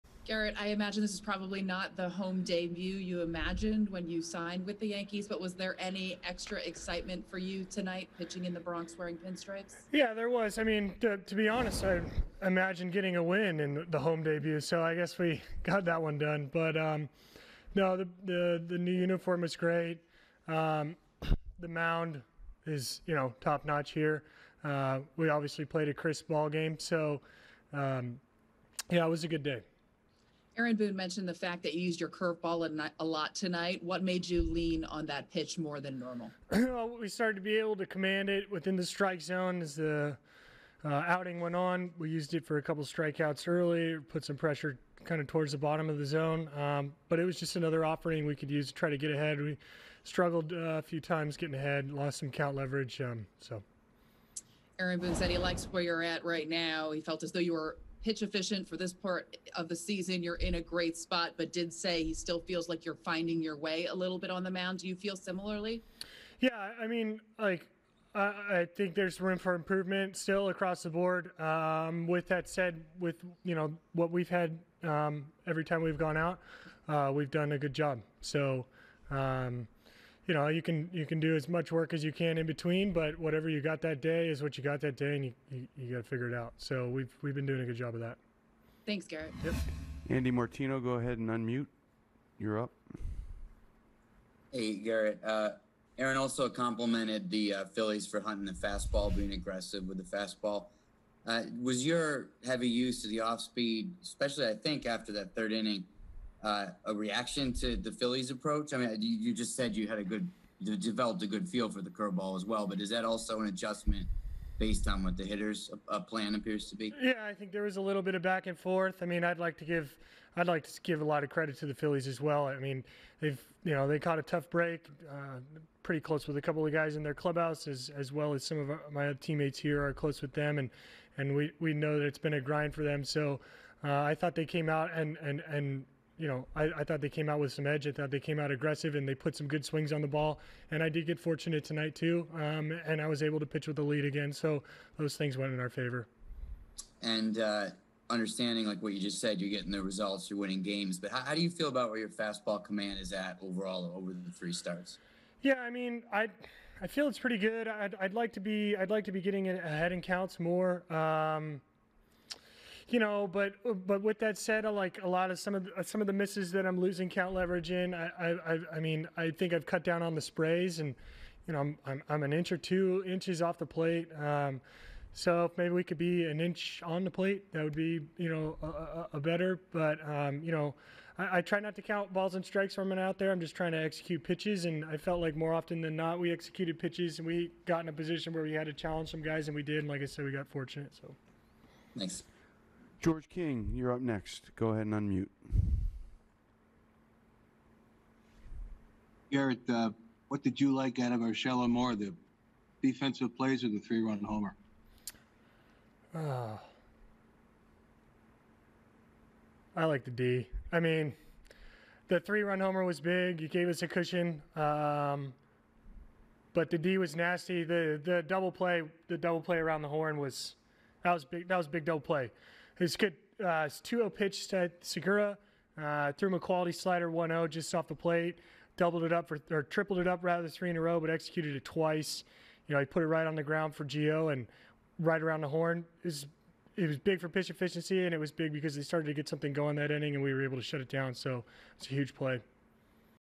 Gerrit Cole breaks down his 2020 home debut
Gerrit Cole moved to 3-0 on the year after tossing six quality innings against the Phillies as the Yankees ace collected his 19th straight regular-season win.